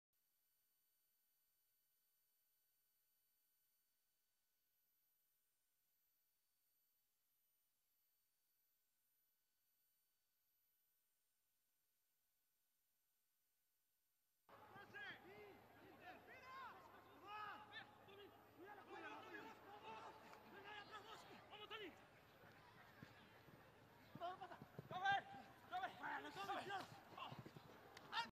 Imparable Tomás Albornoz para Sudamérica XV
Gran corrida de Tomás Albornoz para SudamericaXV ante @RugbyUruguay , falló el pase final 👏👏👏💪🏉